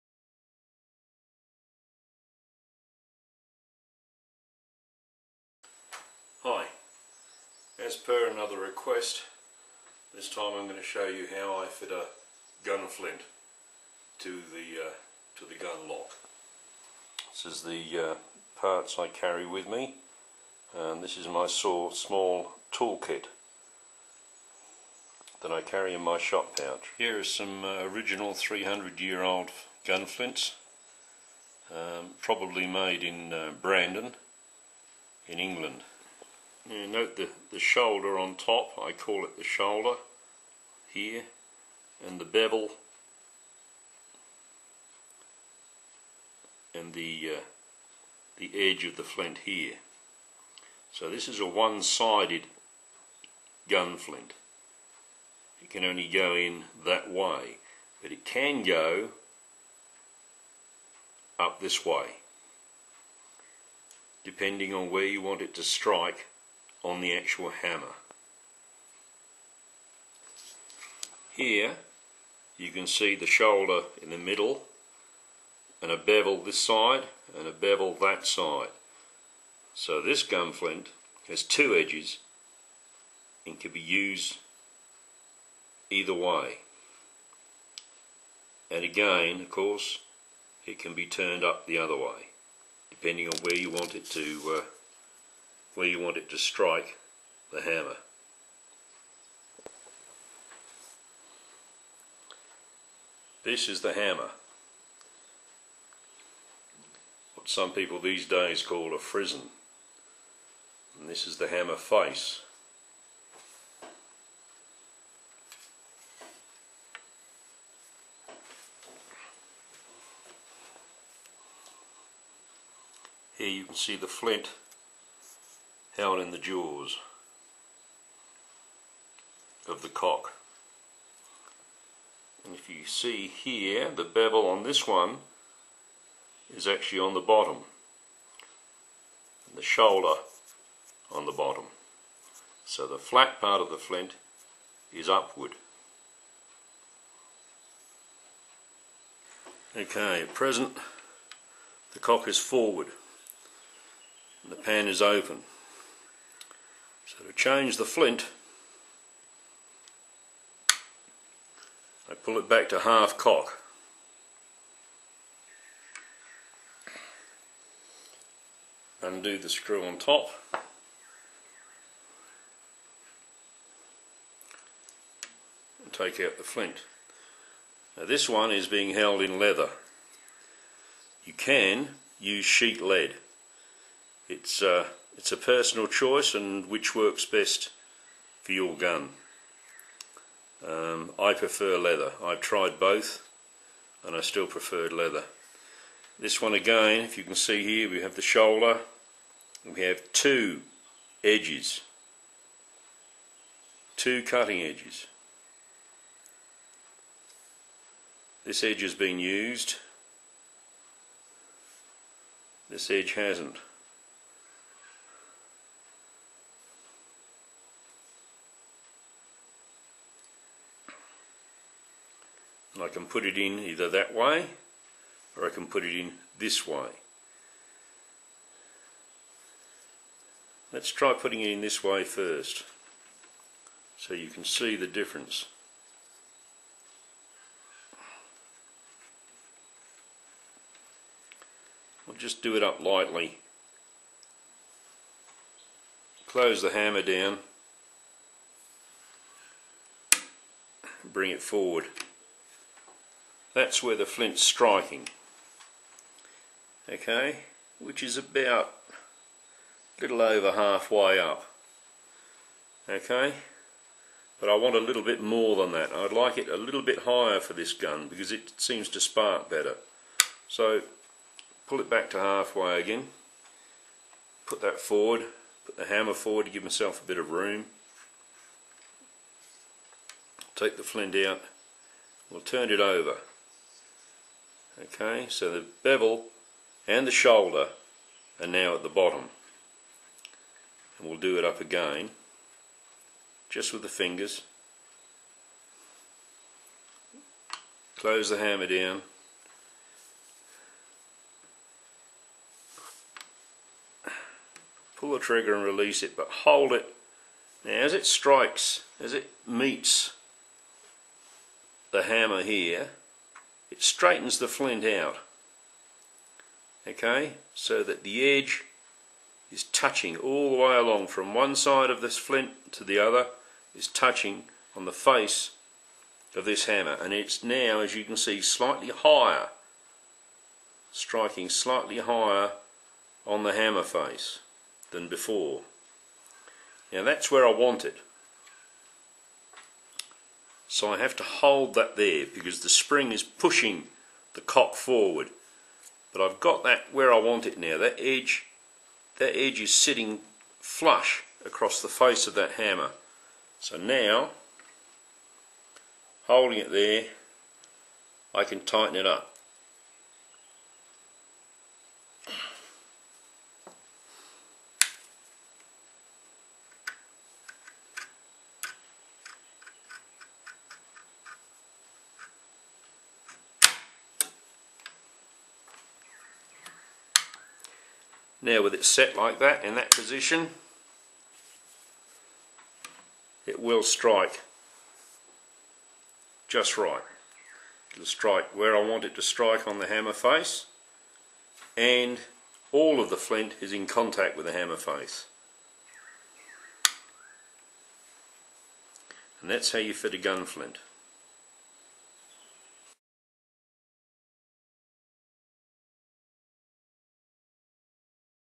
Fitting A Gun Flint In The Lock DVD.
The author demonstrates how he fits a gun flint in the lock.